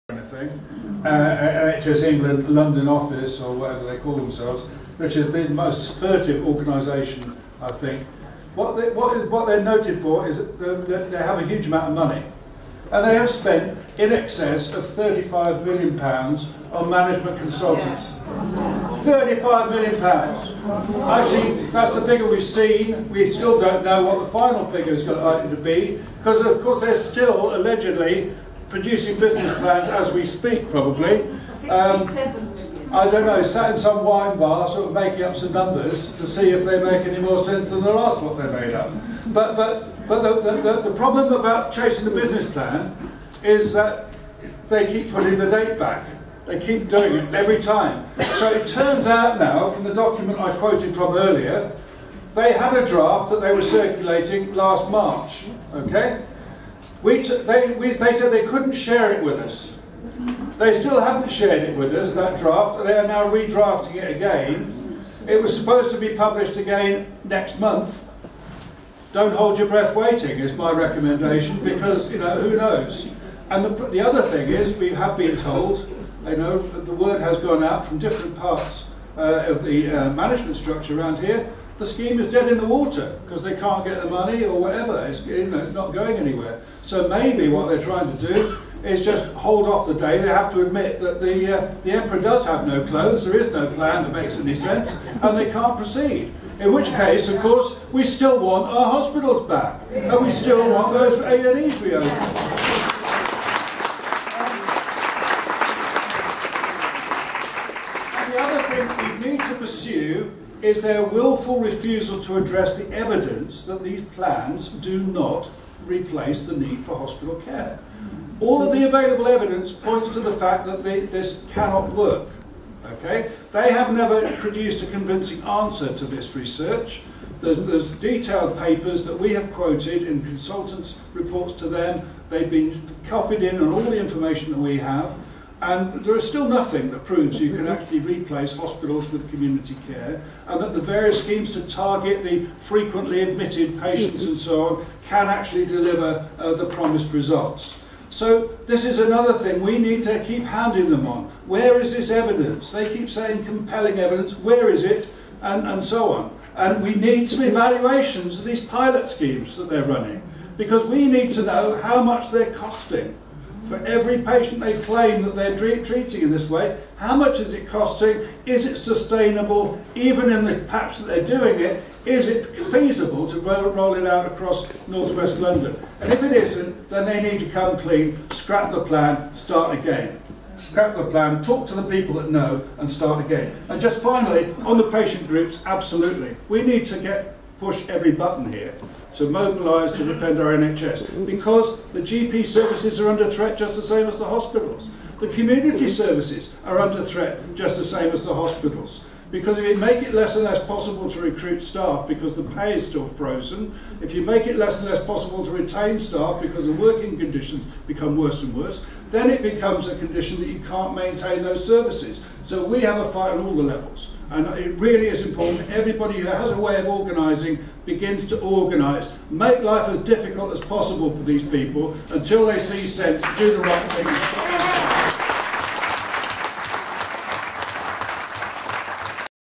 "NHS England spent 35 million on management consultants..." - Dr John Lister
Mansfield report  meeting at Hammersmith Town Hall 23/02/2016

“Deeply flawed hospital plans must be halted immediately” says Michael Mansfield QC

Landmark report says plans threaten fundamental principles of the NHS and provide “no realistic prospect of achieving good quality accessible healthcare for all.”
One of the country’s biggest hospital shake-ups is deeply flawed, likely to exacerbate a deteriorating situation, a threat to the fundamental principles of a universal NHS and should be halted immediately, according to a landmark report from the Independent Healthcare Commission chaired by Michael Mansfield QC.
NHS North West London’s Shaping a Healthier Future (SaHF) programme has already seen the hugely controversial closures of two A&E departments, at Hammersmith and Central Middlesex hospitals and the closure of Ealing maternity unit. Further downgrading of Ealing hospital is planned, along with the closure and sale of the majority of Charing Cross hospital site.
The Independent Commission also uncovered shocking details of spiralling management and consultancy costs. At the same time, a crisis is developing in emergency services, with GP services clearly failing to meet demand across the region, contributing to a crisis in A&E performance.